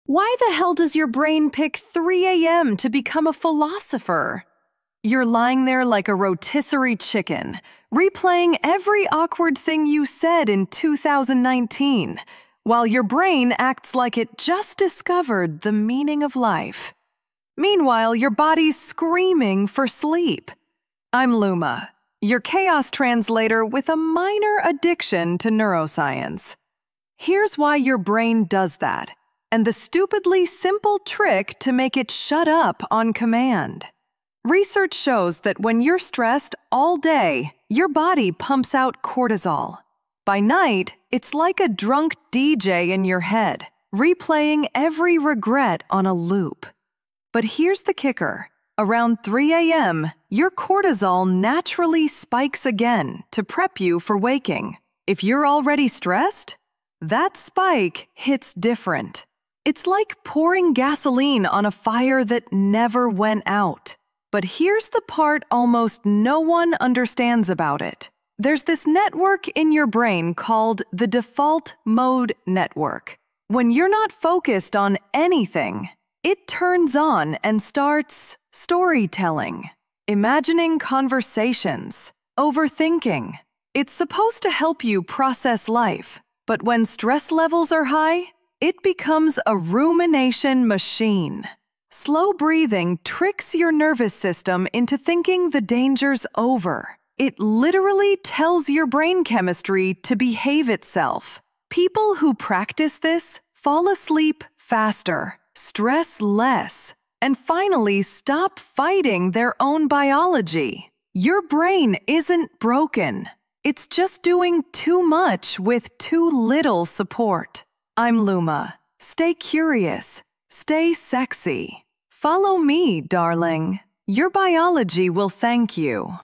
Stop your 3 AM anxiety spiral! 🤯
Stop your 3 AM anxiety spiral! 🤯 Neuroscientist explains why your brain wakes you up and the 1 simple trick to shut down the "drunken DJ" in your head.

That feeling of replaying every embarrassing moment from 2019 at 3 AM? It's not just you! 😩

Neuroscience breaks down why: Stress causes a cortisol pump all day, and around 3 AM, a natural cortisol spike preps your body for waking. If you're already stressed, that spike becomes an anxiety amplifier. Plus, the Default Mode Network (your brain's storyteller) switches on when you're not focused, turning into a "rumination machine."

The Simple Trick: Slow breathing. It literally tricks your nervous system into thinking the danger is over, telling your brain chemistry to behave itself.

Listen to Luma, your chaos translator, for the full explanation and start sleeping better tonight!